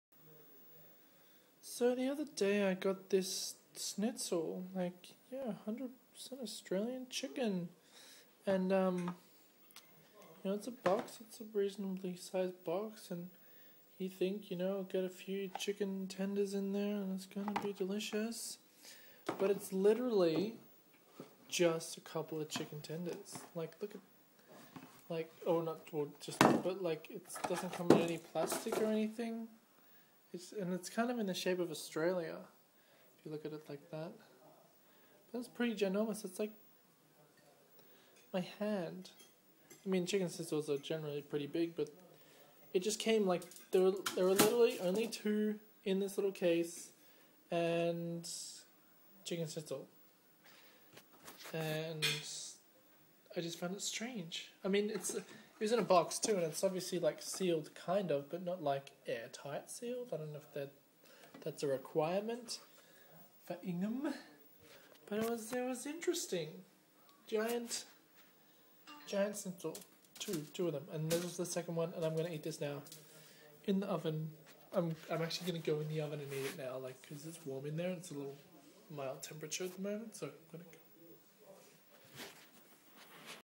Ingham Chicken Schnitzel Packaging
Since when does chicken come without any kind of sealed packaging?